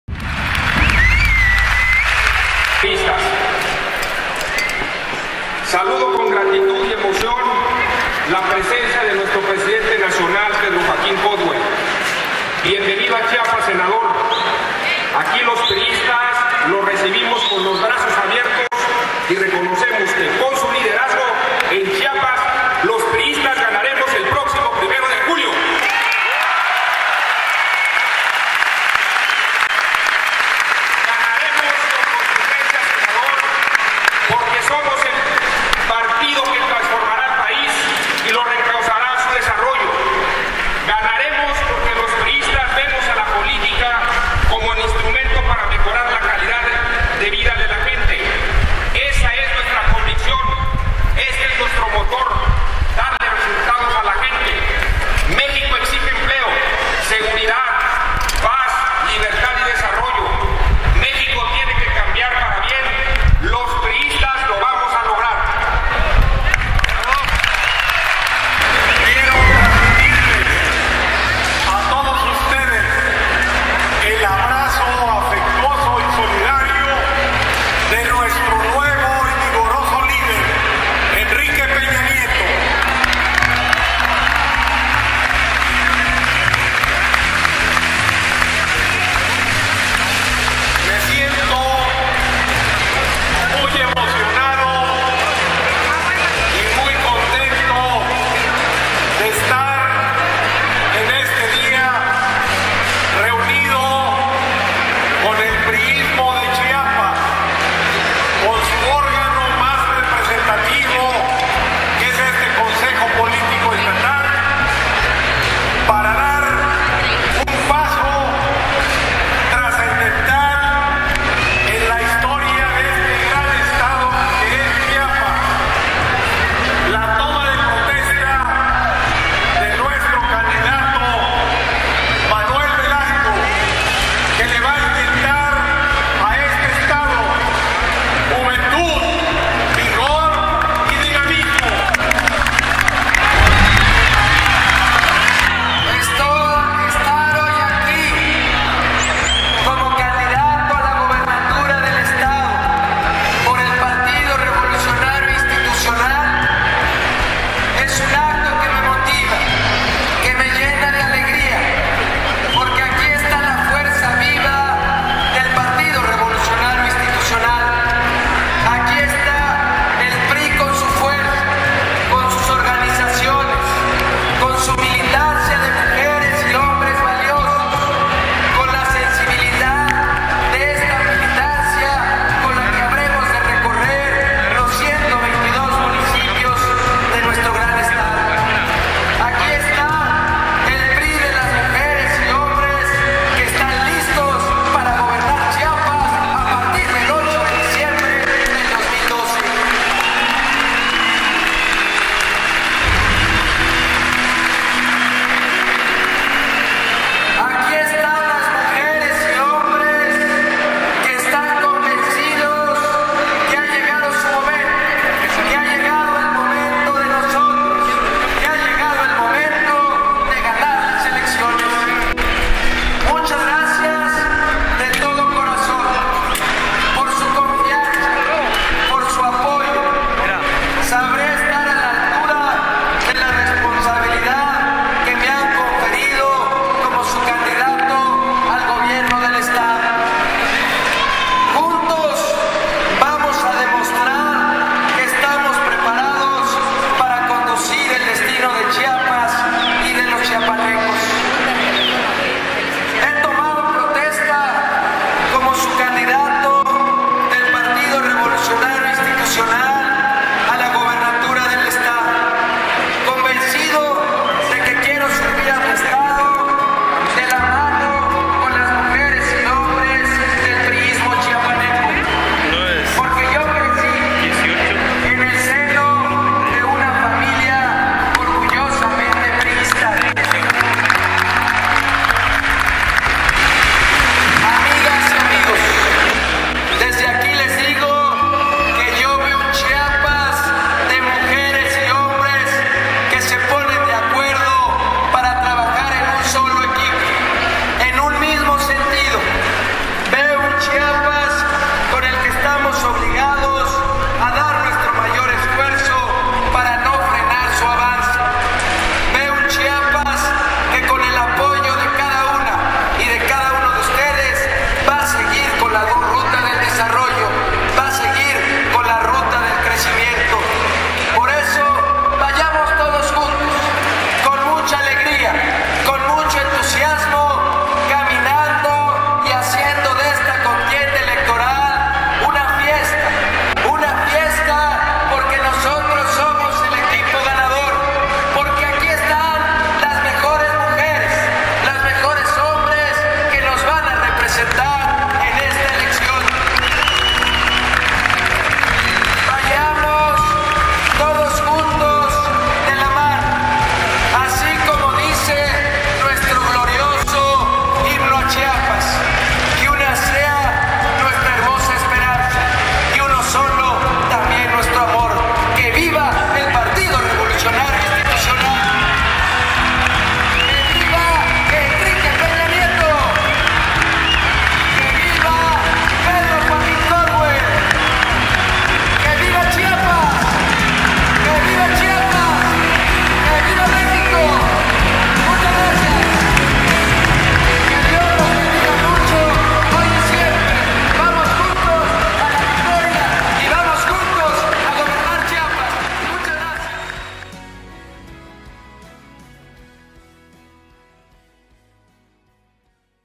VAMOS UNIDOS - Equipo Ganador
Palabras del Presidente Estatal del PRI - Roberto Albores Gleason, Presidente del Comite Ejecutivo Nacional del PRI - Pedro Joaquín Coldwell y del candidato a la gubernatura -  Manuel Velasco Coello el dia 20 de mayo.